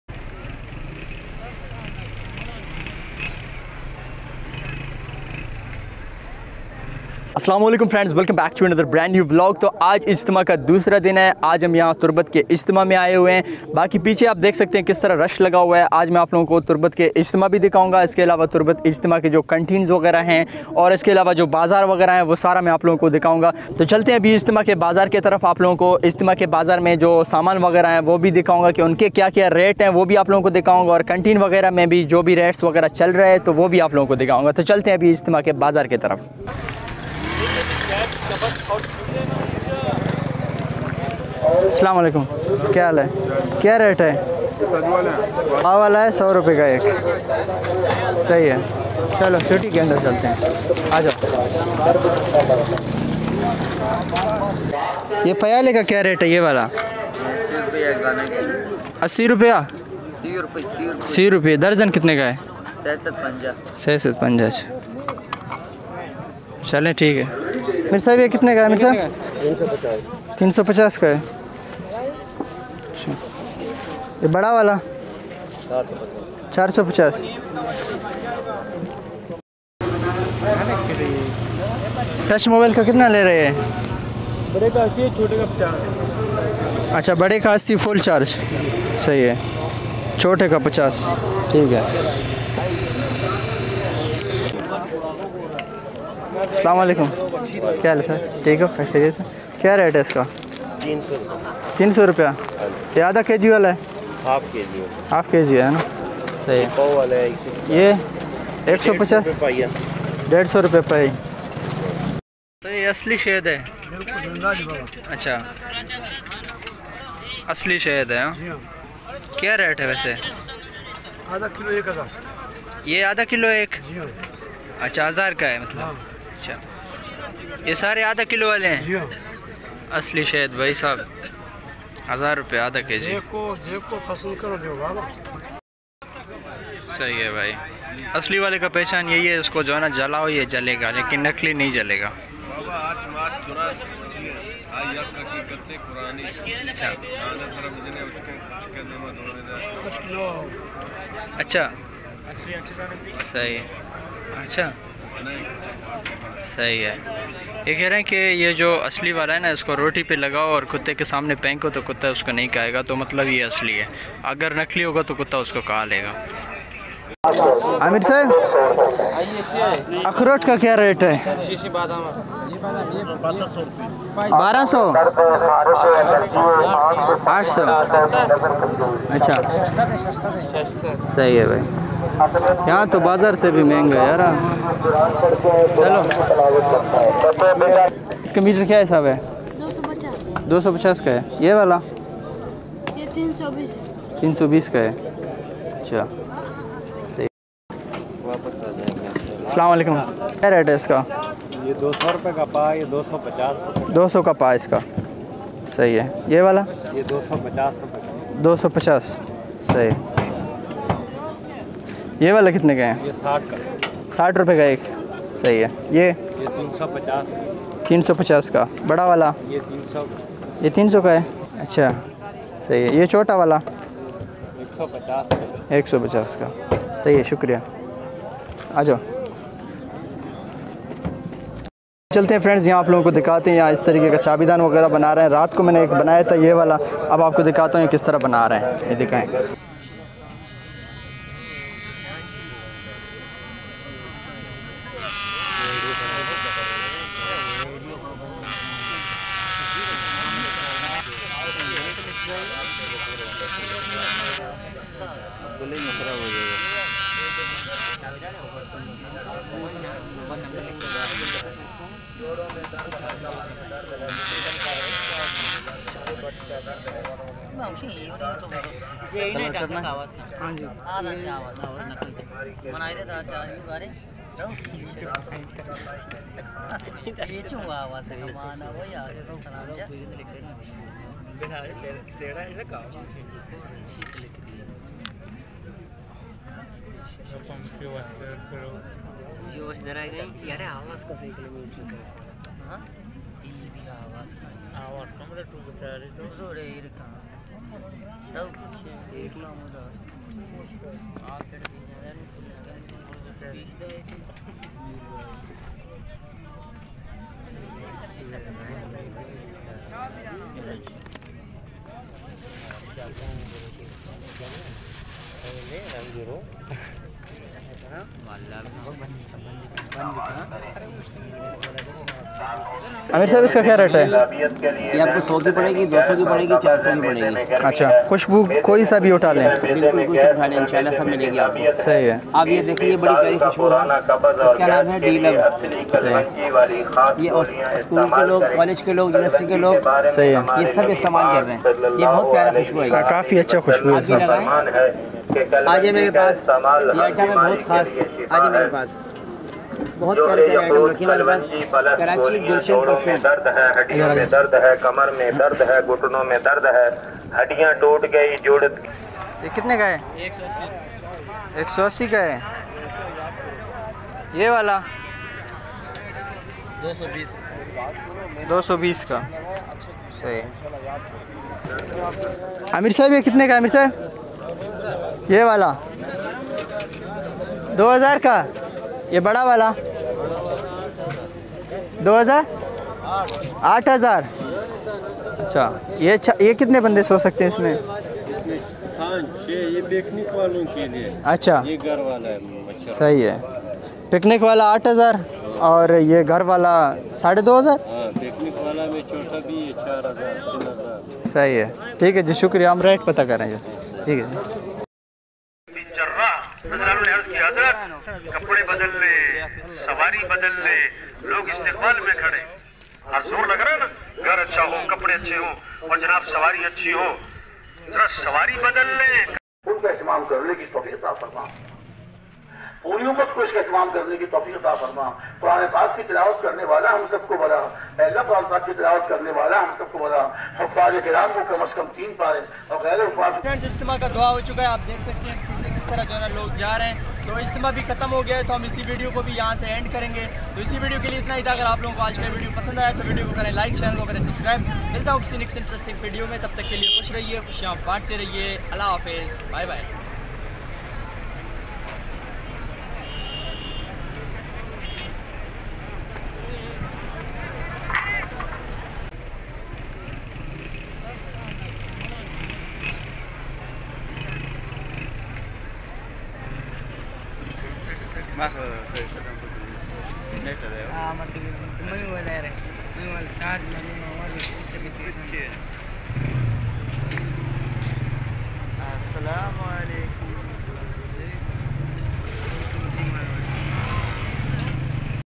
Turbat Ijtema | Turbat Ijtema Bazaar | تربت اجتماع | turbat Ijtema 2022
Turbat Ijtema | Turbat Ijtema  Bazaar Vlog

Assalamualaikum Everyone in this Video I'm going to visit Turbat Ijtema 2022
Turbat Ijtema City Complete Vlog We will share The rates of things being Sold in Turbat Ijtema 2022 what are there prices
Turbat Ijtema Bazaar turbat ijtema
turbat ijtema 2022 تربت اجتماع
ijtema 2022